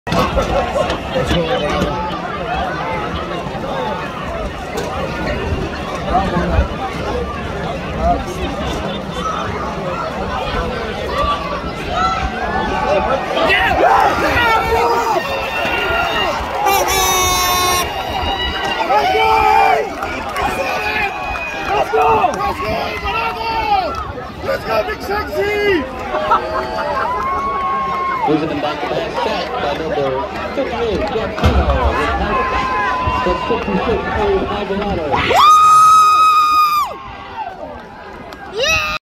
#66 Boylan sophomore, Abraham Alvarado gets through to Hononegah player
#66 Boylan sophomore, Abraham Alvarado gets through to Hononegah player 09/24/2021. Bigsexy AbrahamAlvarado Latino Football Boylantitan Rockford Titanpride #815